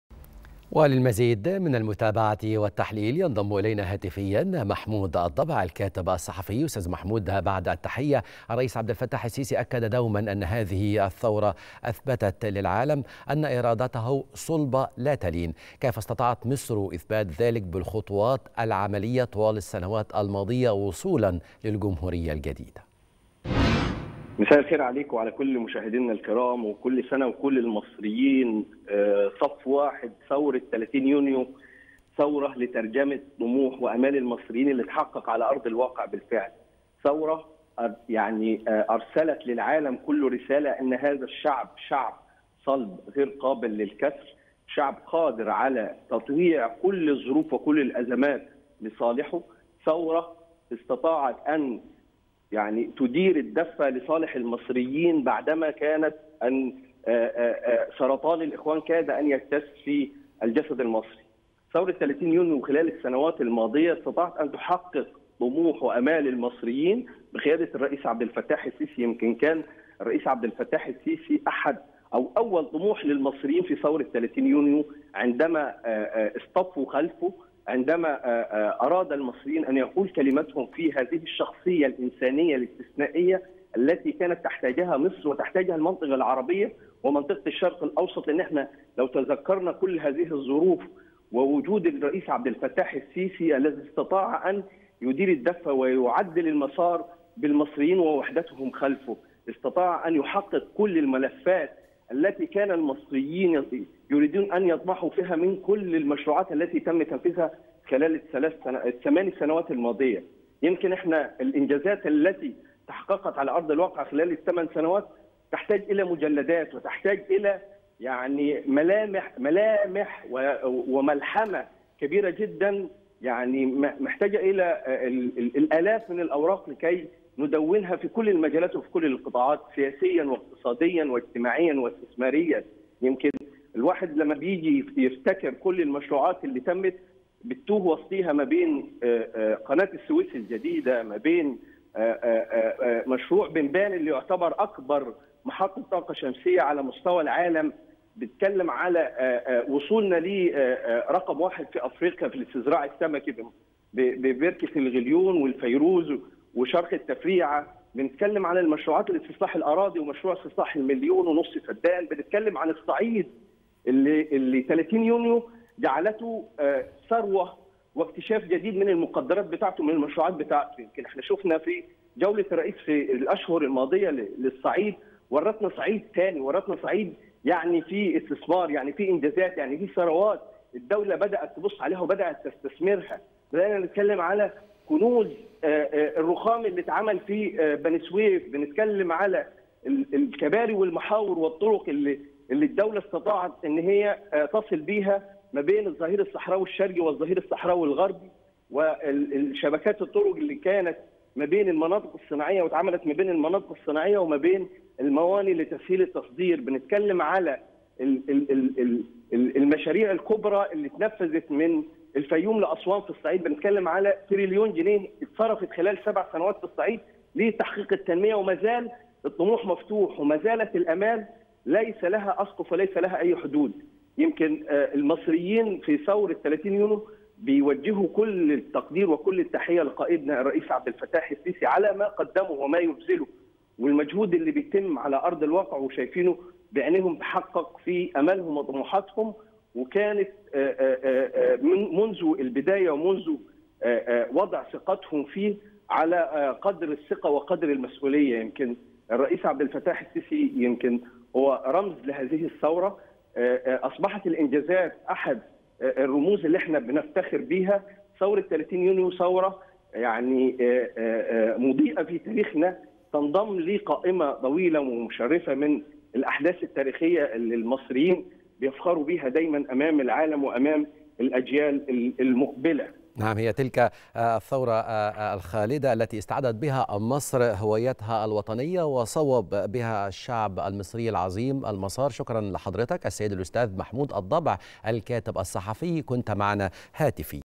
الكاتب الصحفي محمود الضبع: 30 يونيو نقطة مضيئة في تاريخنا تنضم إلى قائمة من والأحداث المشرفة
نشرة الثالثة | الكاتب الصحفي محمود الضبع: ثورة 30 يونيو مضيئة في تاريخنا تنضم إلى قائمة من الإنجازات والأحداث المشرفة